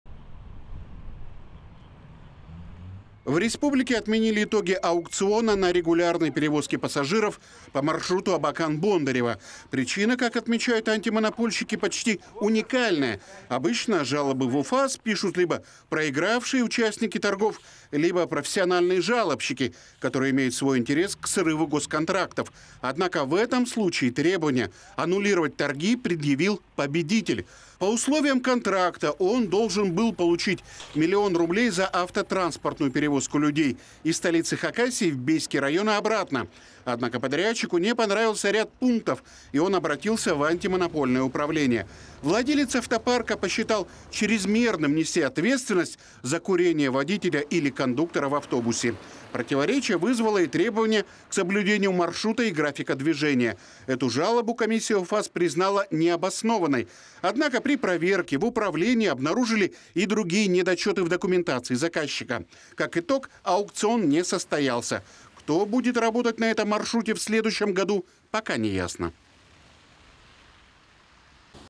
В Хакасии пассажироперевозчик не захотел отвечать за курящих водителей
В Хакасии пришлось отменить итоги аукциона на регулярные перевозки пассажиров по маршруту №553  Абакан - село Бондарево в 2022 г.. Заказчику - Минтрансу Хакасии - предстоит переделать аукционную документацию и провести закупку снова.

 157-километровый маршрут включает Абакан, Белый яр, Сартыков, Х,Салда, Аршаново, Шалгинов, Чаптыков, Куйбышево, Уты, Бея, Буденовка, Табат, В.Киндирла, Бондарево.